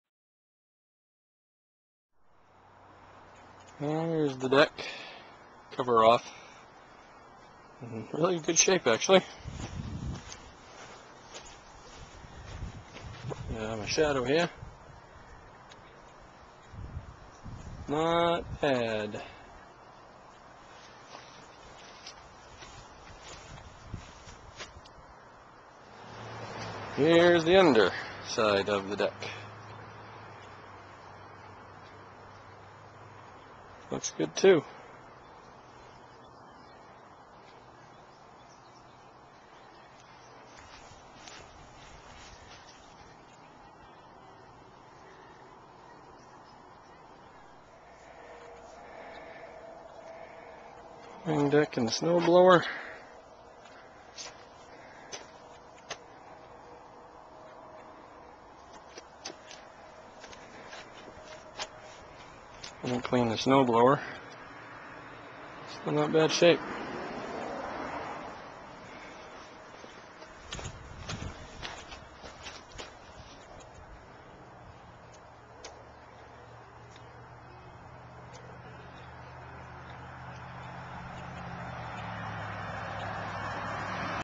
Wheel Horse Bronco Mowing Deck and Snow Blower
Here's the mowing deck and snow blower that go with the 1971 Wheel Horse Bronco 14